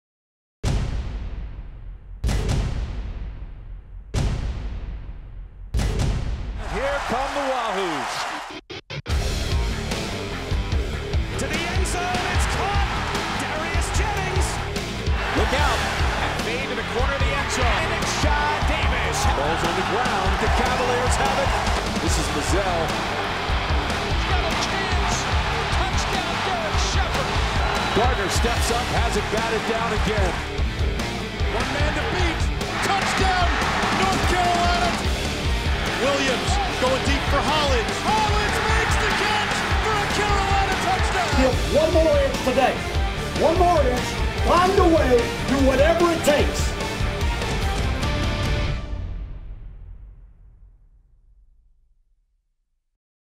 UNC vs UVA Hype Video
It's a battle of two coastal division foes as North Carolina travels to Charlottesville, VA to take on the Virginia Cavaliers. Take a look at the best of the best from both teams and their fans to get hyped up for Saturday's match-up!